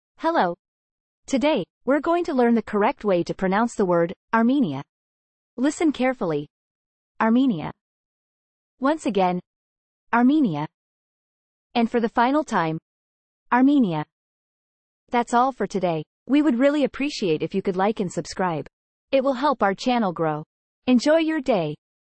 How to Pronounce "Armenia"
This video demonstrates how to correctly pronounce the word: Armenia.